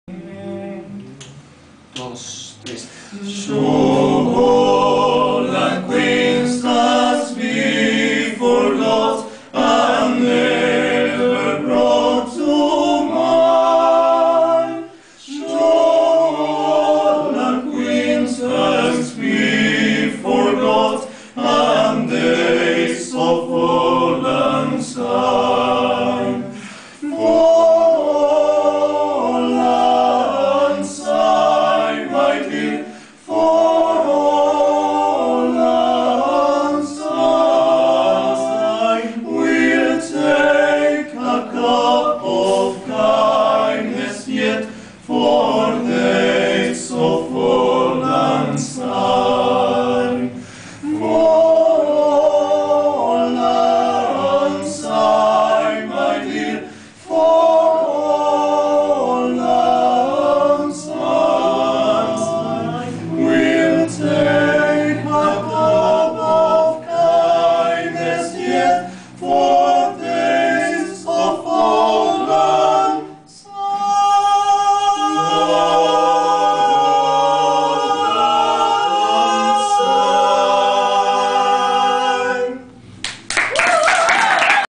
Primer Tetra-Concierto - 3
Presentación del Primer Concierto del grupo Vocal "Tetra".
Integrantes: Pablo, Alejo, Alfredo y Enzo.